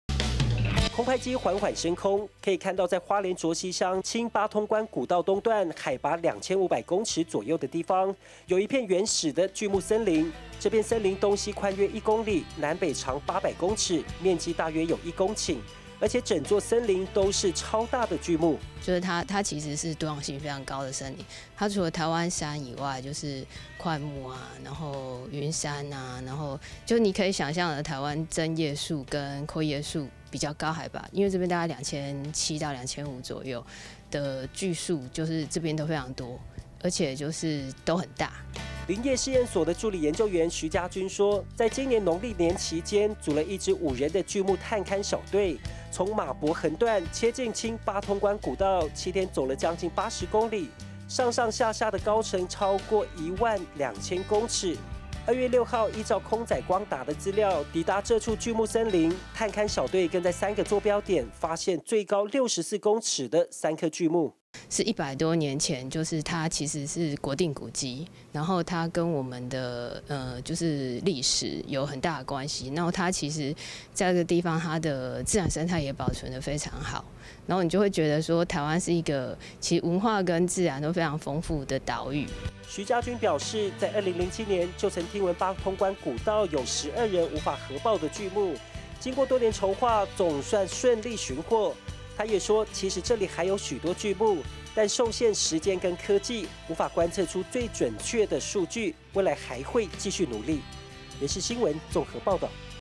清八通關古道 發現約1公頃"巨木森林" 2019-02-19 IPCF-TITV 原文會 原視新聞
山林裡的新發現、農委會林業試驗所的研究員，在今年的2月深入中央山脈，前往清八通關古道，發現了一處面積大約一公頃的原始巨木森林，在高海拔的山裡相當罕見。

這支5人的探勘小隊，是花了7天時間，走了將近80公里，最後更找到3棵超過60公尺的巨木。

空拍機緩緩升空，可以看到在花蓮卓溪鄉，清八通關古道東段，海拔2500公尺左右的地方，有一片原始的「巨木森林」；這片森林東西寬約1公里，南北長800公尺，面積大約有1公頃，而且整座森林都是超大的巨木。

林業試驗所的助理研究員徐嘉君說，在今年農曆年期間，組了一支5人的巨木探勘小隊，從馬博橫斷，切進清八通關古道，7天走了將近80公里，上上下下的高程超過1萬2千公尺，2月6號依照「空載光達」的資料，抵達這處巨木森林。

探勘小隊更在3個座標點，發現最高64公尺的3棵巨木。

徐嘉君表示在2007年，就曾聽聞八通關古道有12人無法合抱的巨木，經過多年籌畫，總算順利尋獲；她也說其實這裡還有許多巨木，但受限時間與科技，無法觀測出最準確的數據，未來還會繼續努力。